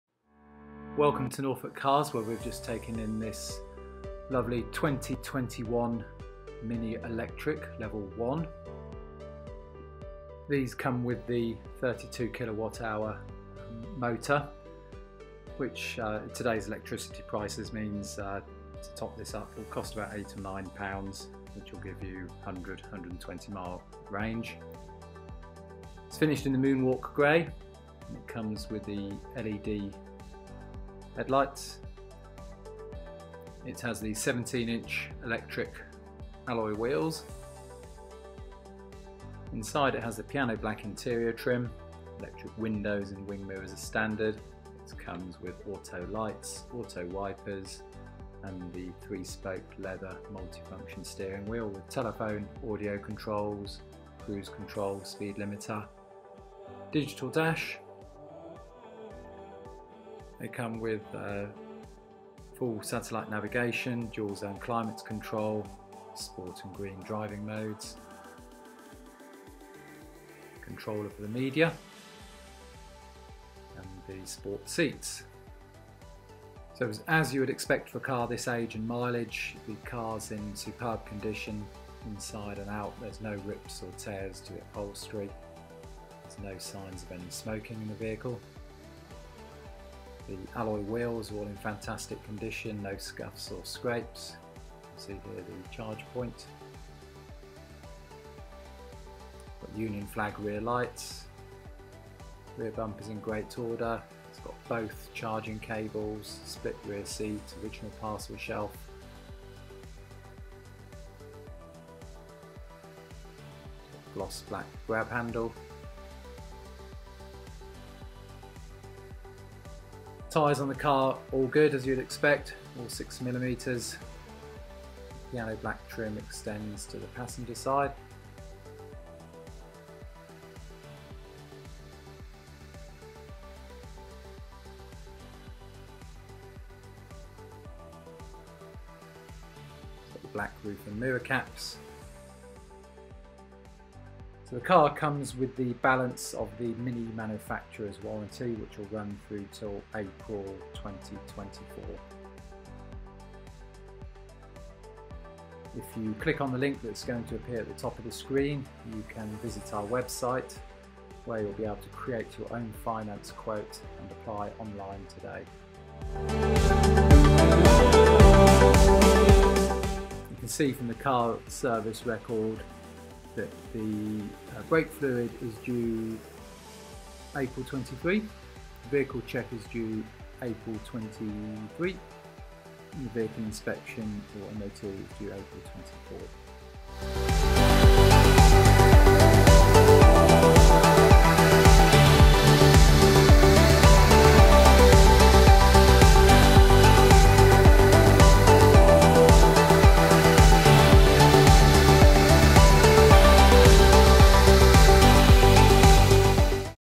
2021 Mini Electric Level 1 Grey
2021 Mini Electric Level 1 Grey for sale at Norfolk Cars Mini Specialist.  Satellite Navigation, Climate control, Digital dash.